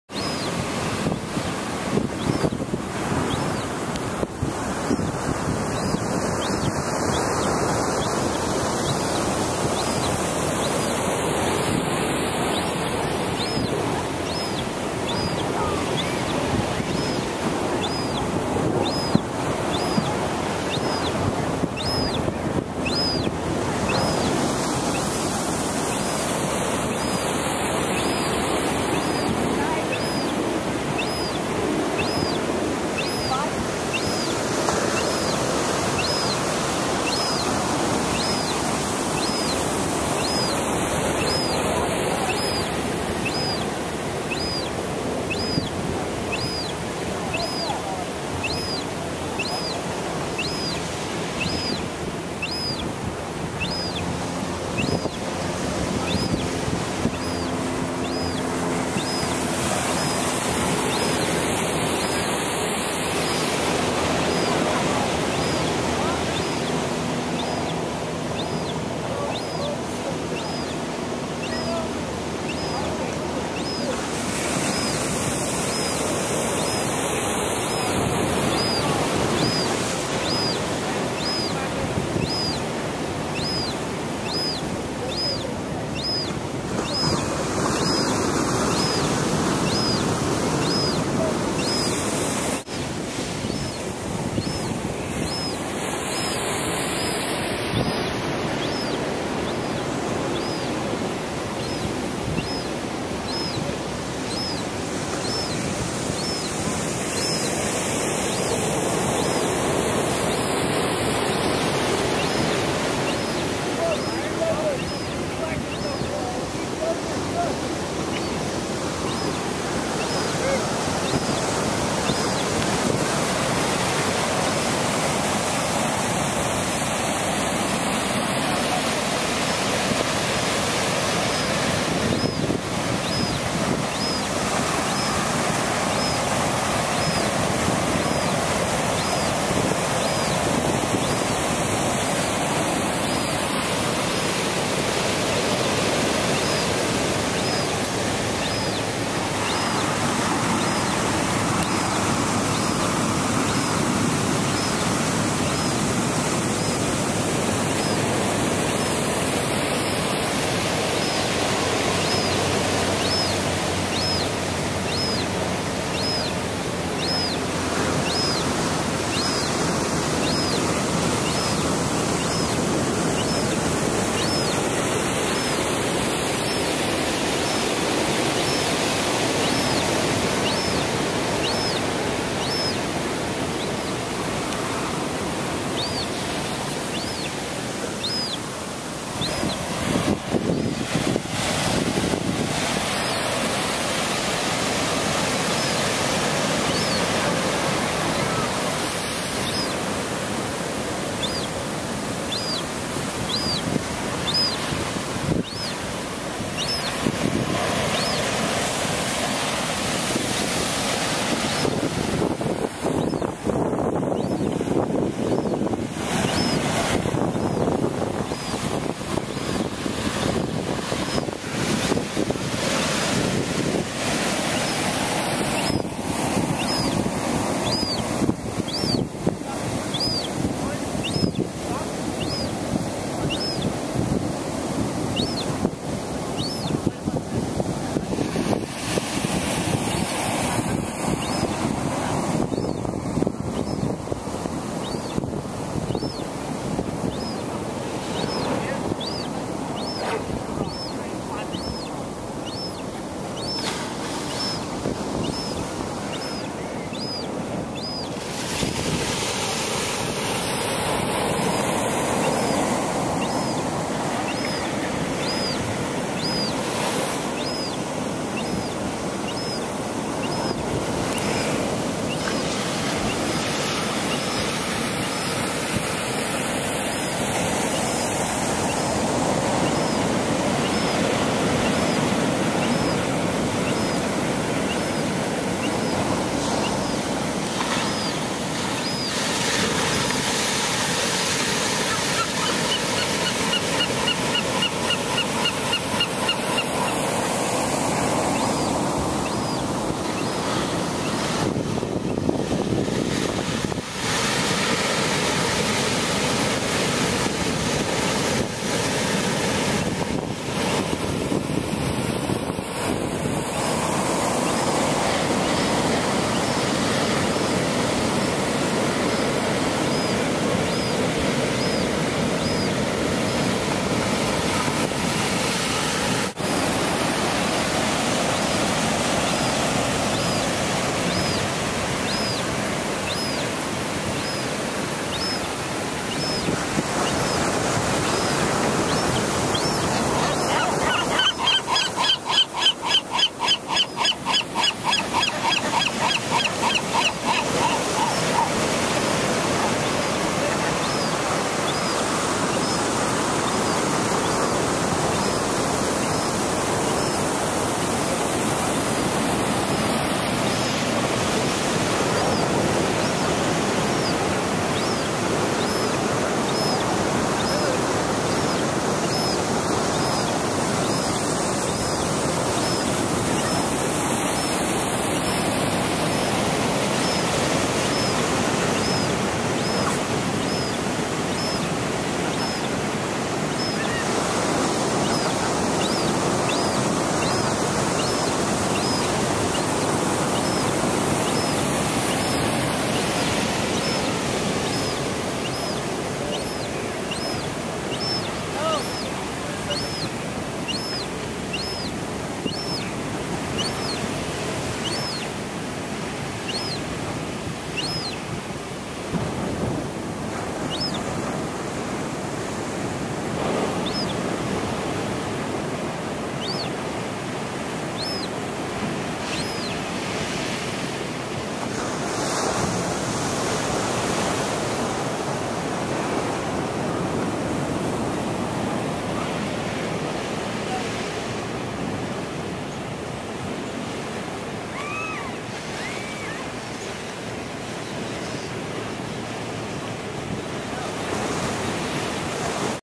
Baby Seagull Cries for Food in La Jolla & Parents Fly Away
Baby Seagull Cries for Food in La Jolla Parents Fly Away. The baby Bird follows his Momma around crying for food. The Momma Bird walks away and the baby bird follows her. The Momma Bird flies away and the baby bird follows her still. Eventually the Poppa bird flies back and then both birds fly away leaving the baby seagull crying, hungry & alone.